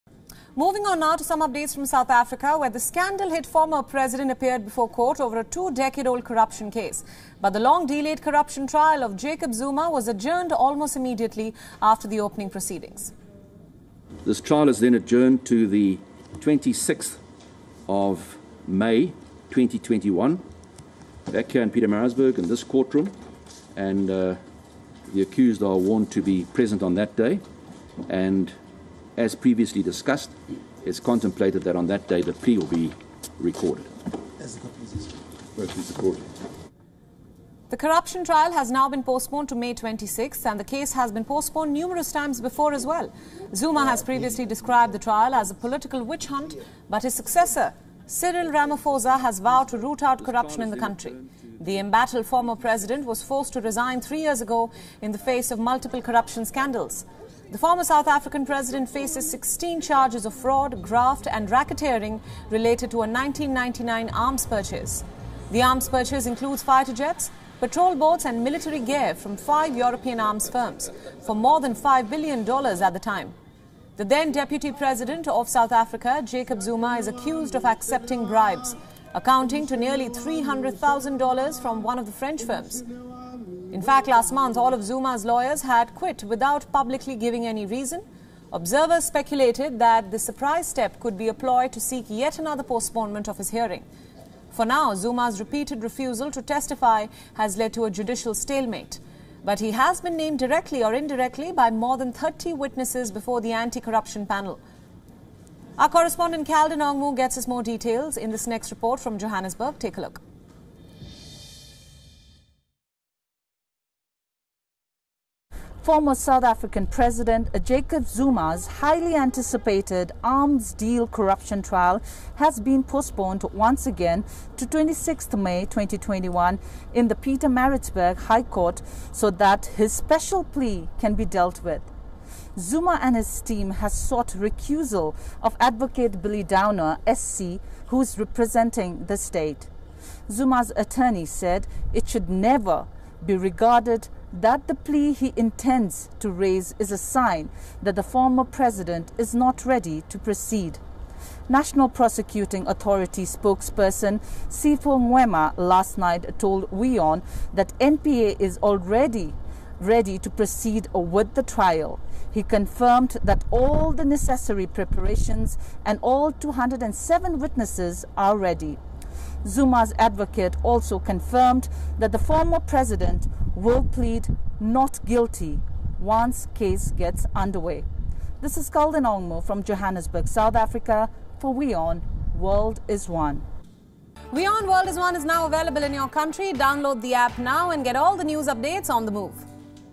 South Africa: Jacob Zuma corruption trial postponed to May 26| Latest World News | WION English News
South Africa's Former president Jacob Zuma appeared before the court over a two-decade long corruption case. But the long-delayed corruption trial of Zuma was adjourned almost immediately after the opening proceedings for nine days.